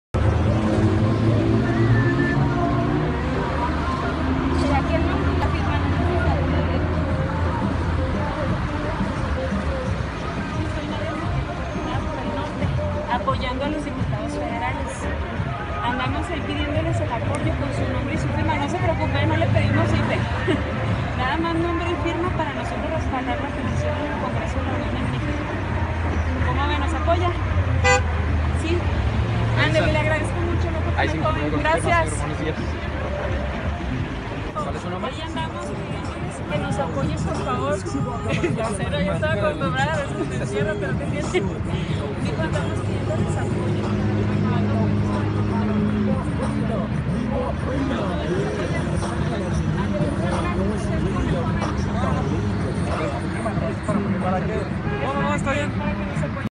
La diputada Nadia Siqueiros, volantea contra "El Gasolinazo"
Chihuahua.- La diputada panista Nadia Siqueiros junto con jóvenes de Acción Nacional, realizaron un volanteo y petición de firmas, a todos los conductores que transitaban por el crucero de la Ortiz Mena y Mirador.